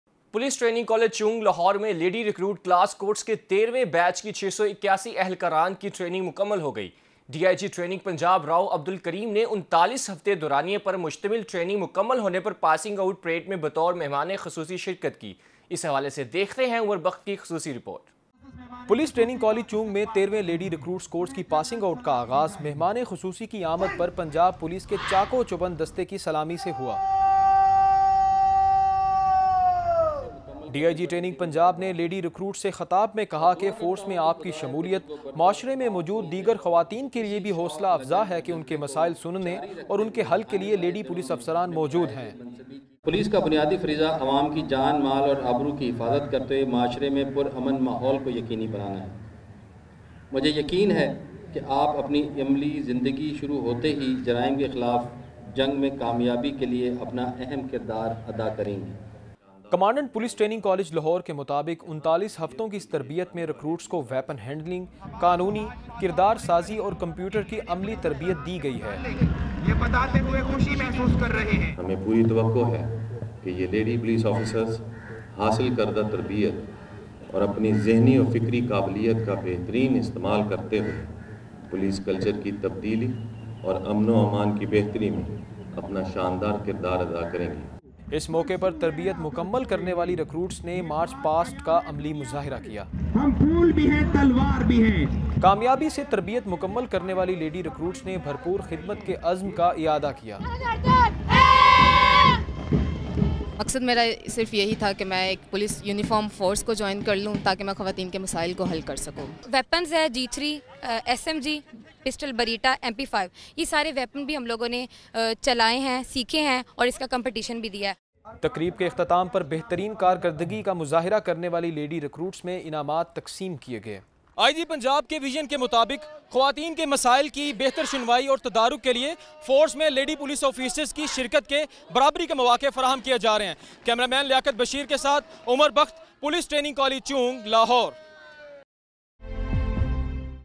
13th Badge Passing Out Parade of Lady Recruit Class Course at Police Training College Lahore
13th Badge Passing Out Parade of Lady Recruit Class Course at Police Training College  Lahore